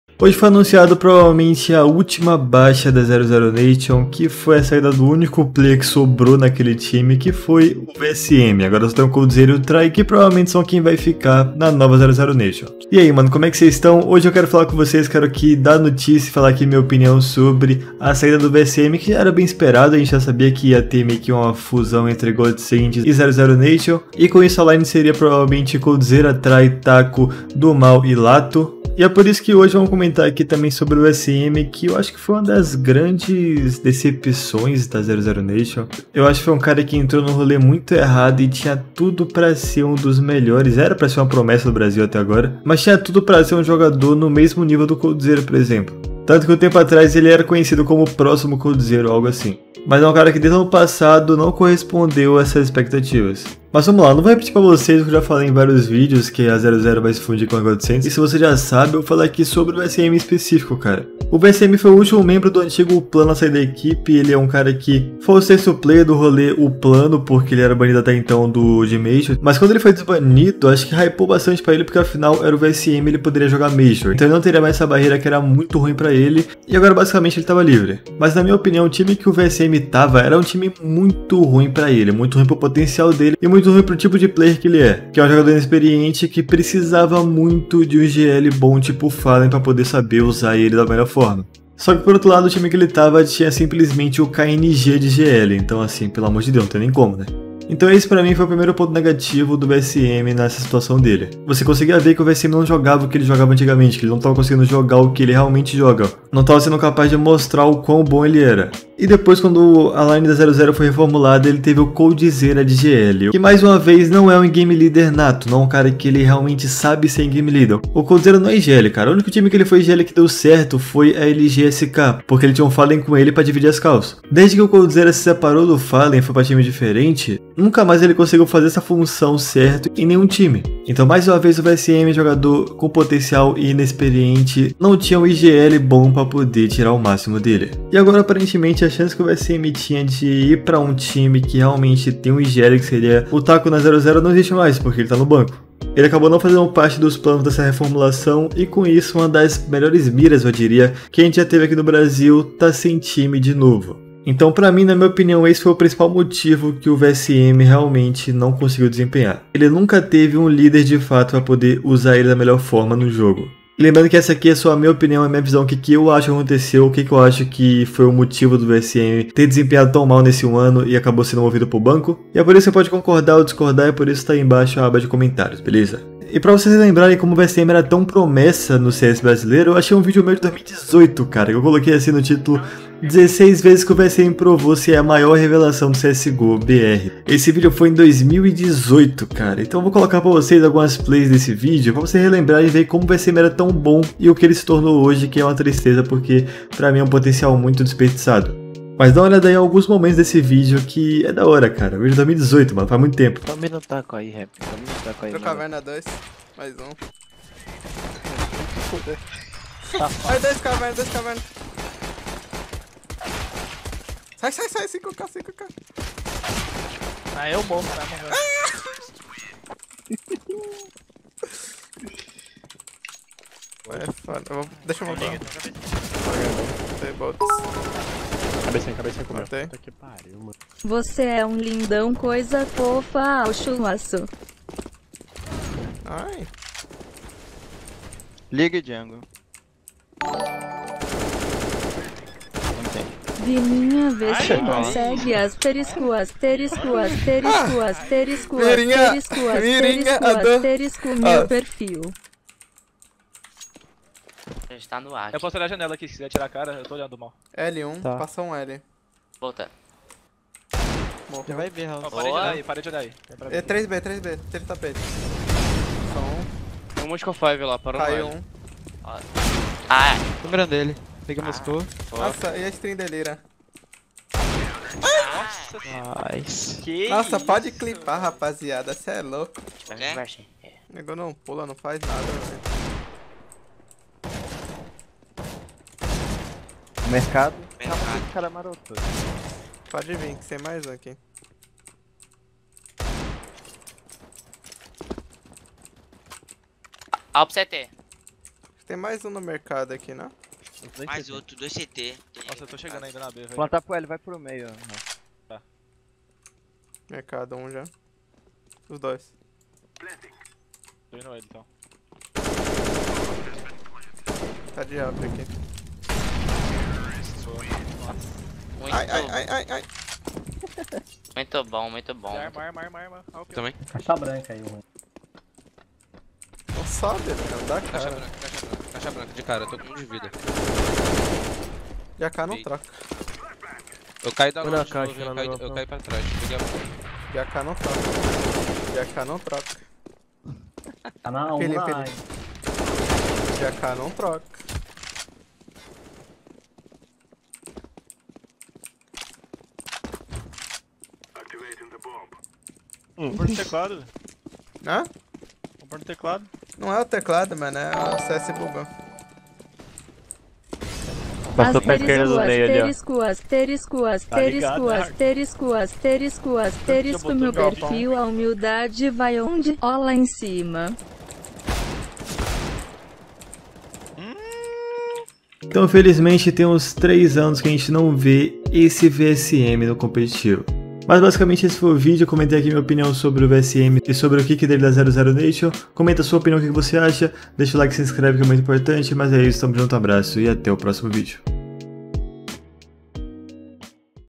O QUE ACONTECEU COM O VSM?
O cara que era uma das maiores promessas do CS:GO BR, hoje ta sem time e no banco da 00Nation

Meu outro canal - @lucasburn